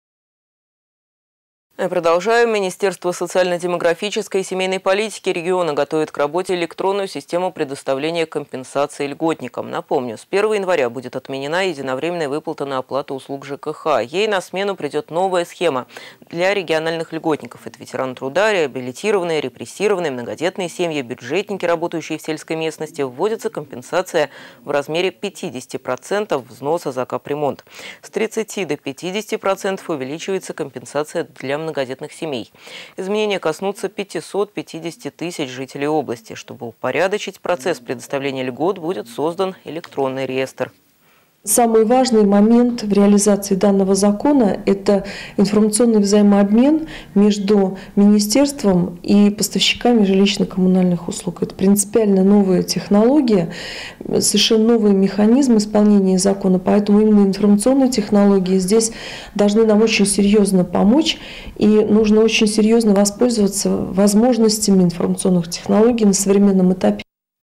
Минсоцразвития готовит к работе электронную систему предоставления компенсаций льготникам
Министерство социально-демографической и семейной политики региона готовит к работе электронную систему предоставления компенсаций льготникам. С 1 января будет отменена единовременная выплата на оплату услуг ЖКХ. Ей на смену придет новая схема. Для региональных льготников вводится компенсация в размере 50 % взноса за капремонт. С 30 % до 50 % увеличивается компенсация для многодетных семей. Изменения коснутся 550 тыс. жителей области. Чтобы упорядочить процесс предоставления льгот, будет создан электронный реестр.